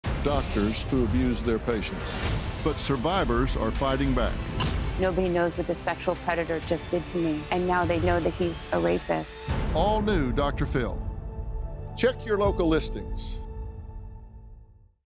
Groomed and Deceived by White Coat Predators
In October 2022, Dr. James Heaps, an obstetrician-gynecologist and oncologist at UCLA, was found guilty of five counts related to sexual assault and misconduct against several female patients. Former patients Melanie, Ellen, Gaby, and Nicole, are speaking out about the horrific abuse they claim they endured. Dr. Heaps is just one of many doctors that work in prestigious universities who have broken their Hippocratic Oath to “do no harm.” Former University of Michigan football player Jon Vaughn shares his story of the abuse he claims he endured from Dr. Robert Anderson. He claims he is just one of thousands of his victims who were abused under the guise of “medical care.” Former gymnast Trinae claims she was one of Dr. Larry Nassar’s first victims of abuse. She claims he sexually abused her for years in plain sight. Judge Rosemarie Aquilina, the Michigan circuit court judge who sentenced Nassar and sent him to prison, says she will continue to fight to put white coat predators where they belong: behind bars. This show contains sexual content.